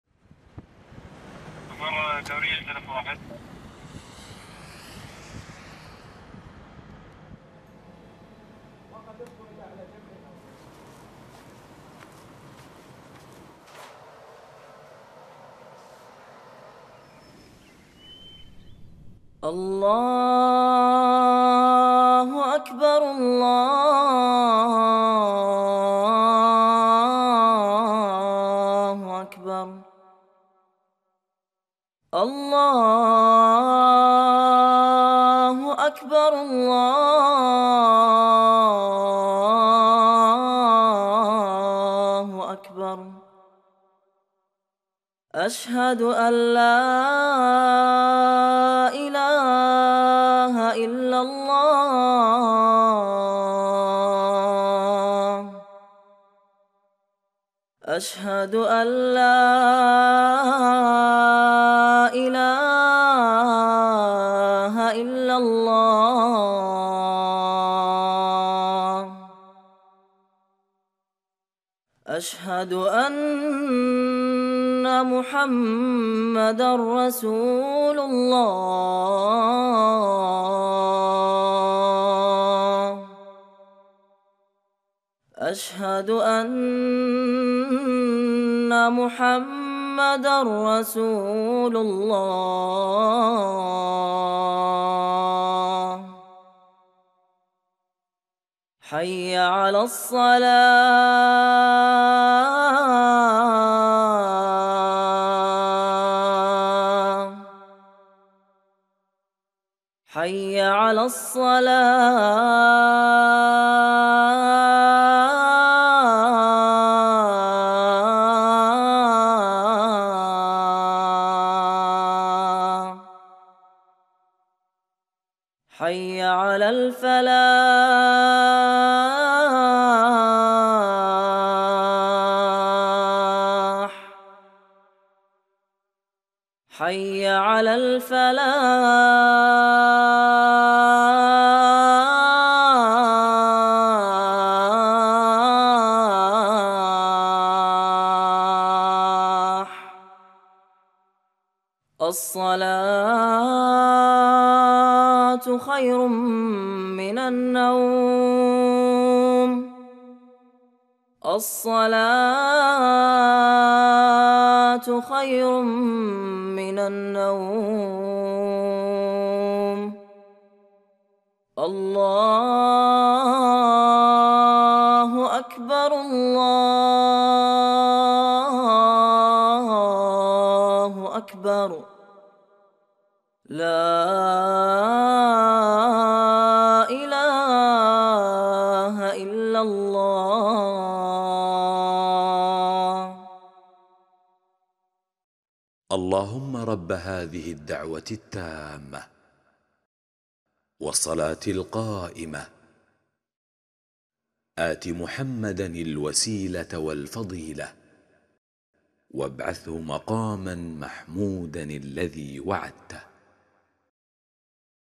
أذان الفجر
ألفاظ الأذان معروفة, وهي خمس عشرة جملة عند الحنابلة والحنفية, وهي المروية من أذان بلال رضي الله عنه  :
اللهُ أكبرْ , اللهُ أكبرْ  . اللهُ أكبرْ , اللهُ أكبرْ
أشهد أن لا إلهَ إلا الله. أشهد أن لا إله إلا الله
أشهد أن محمداً رسولُ الله . أشهد أن محمداً رسول الله
حيِّ على الصلاة . حي على الصلاة
حيِّ على الفلاح . حي على الفلاح
الصلاة خير من النوم . الصلاة خير من النوم .
اللهُ أكبرْ . اللهُ أكبرْ